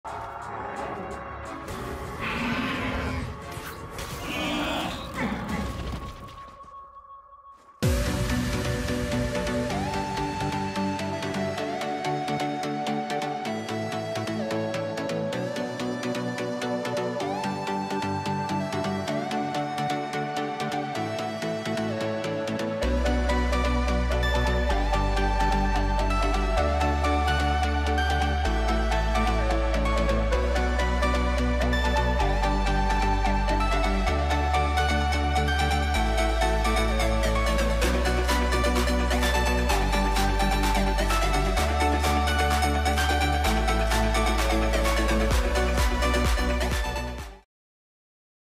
LEGO Star Wars Blurrg MOC
Сделал короткий на мою самоделку ЛЕГО БЛАРРГА